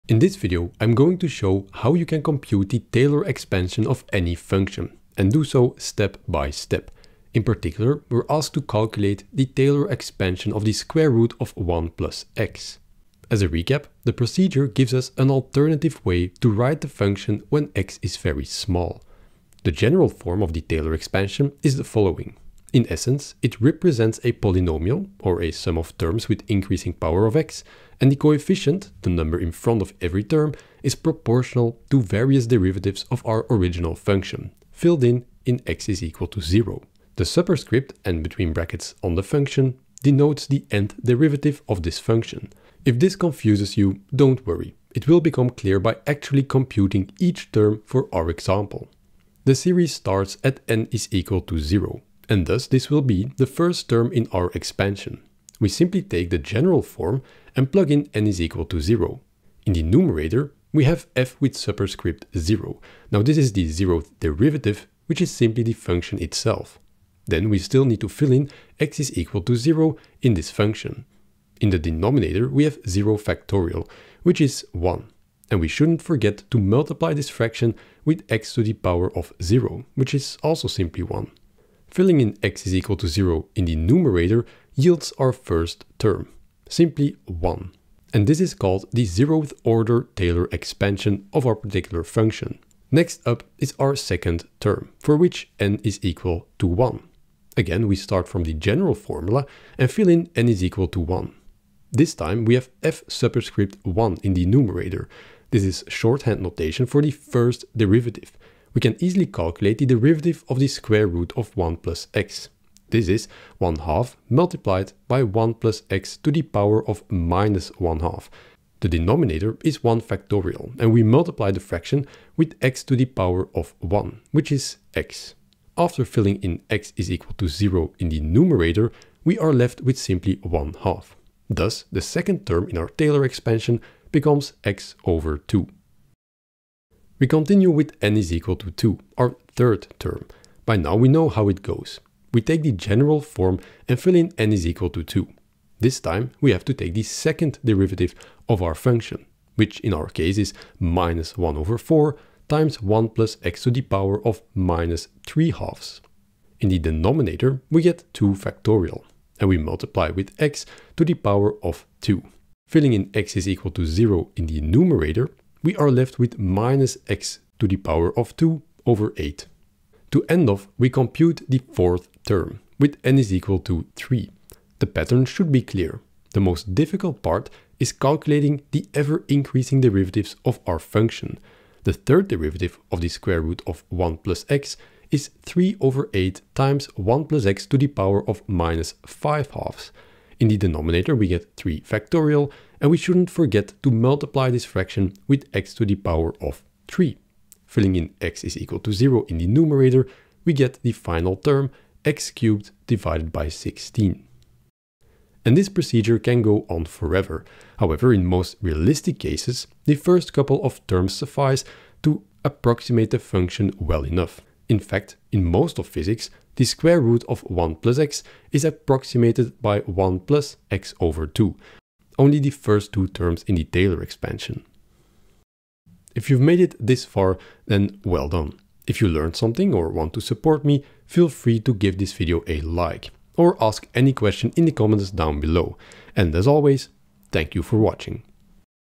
Step-by-Step Calculation of the Taylor Expansion: sqrt(1+x)
📚 How to calculate the Taylor Expansion of sqrt(1+x). This is what we will do in this video, step-by-step. We will compute the first 4 terms in the series.

⏱ Timestamps:
00:00  Intro
00:19  General Formula
00:49  First Term
01:36  Second Term
02:18  Third Term
02:53  Fourth Term